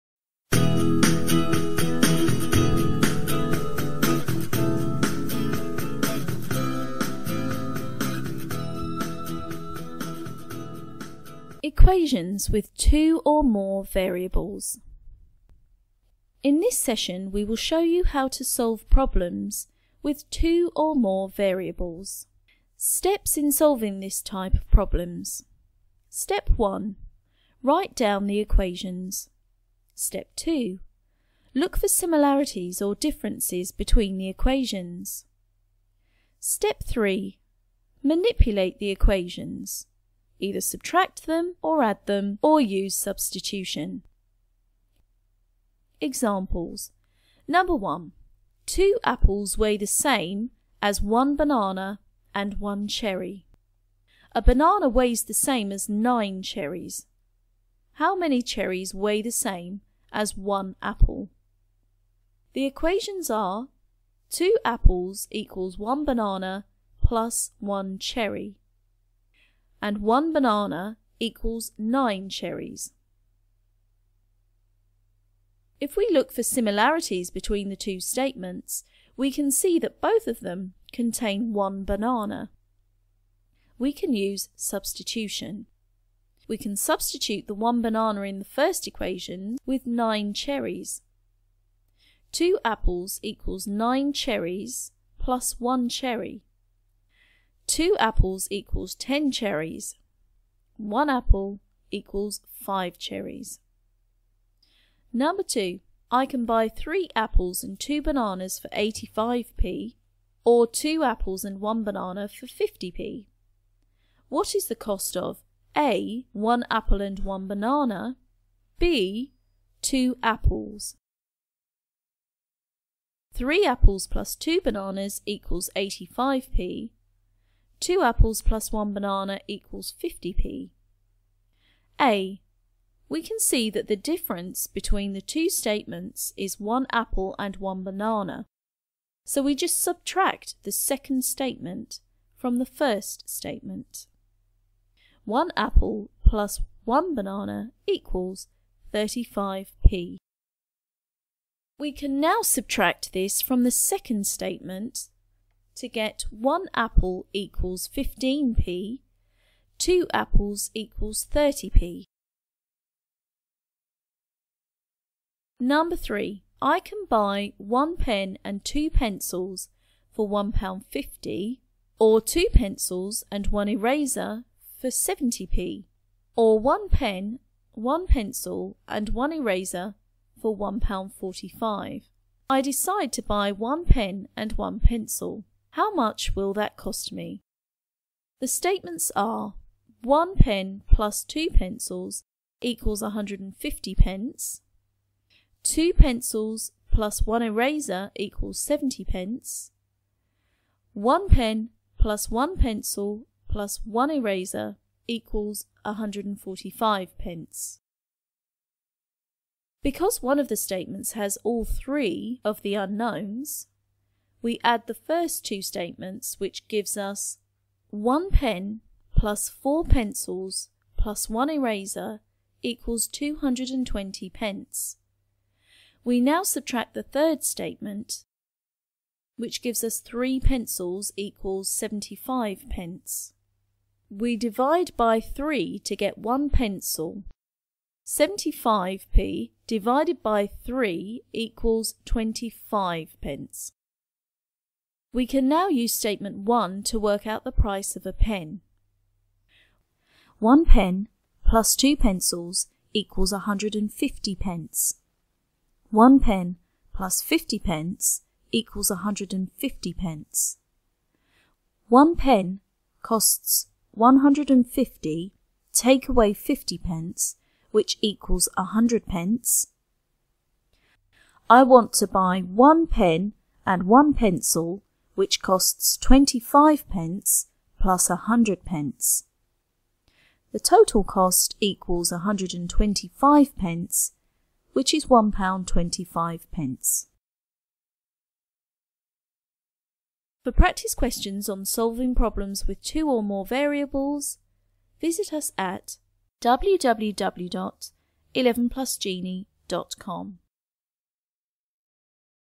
11 Plus Genie Maths – Solve Equations with two or more variables
In this video we show you how to solve problems with two or more variables (unknowns) in.